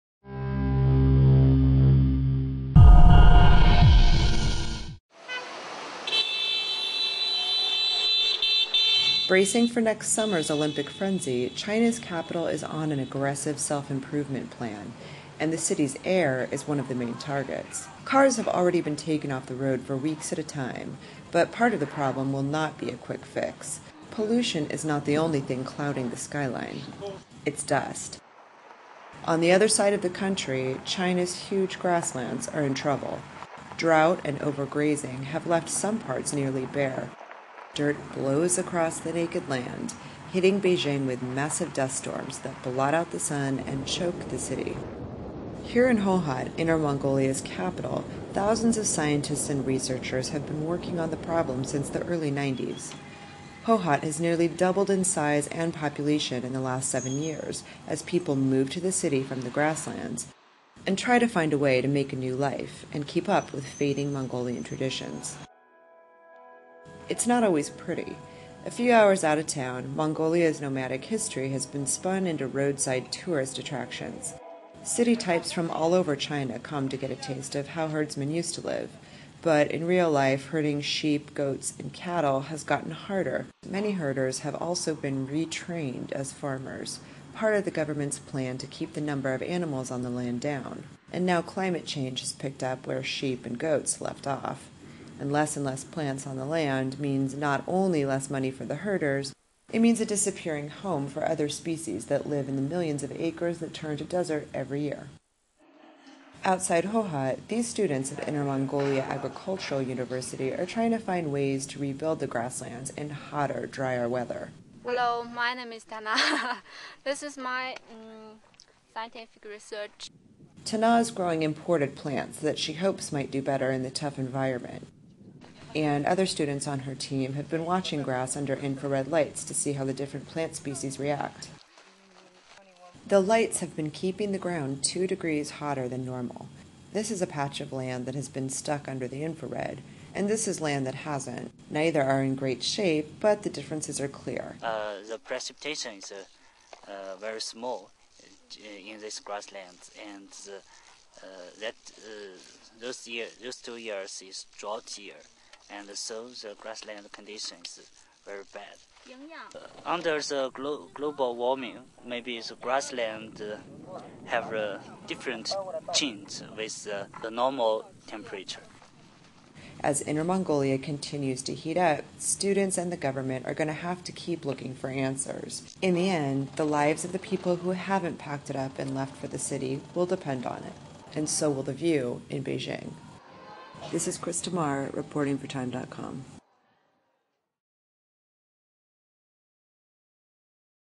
Saving China's Grasslands | TIME
Scientists are working to save grasslands as desertification in parts of northern China has accelerated in recent years.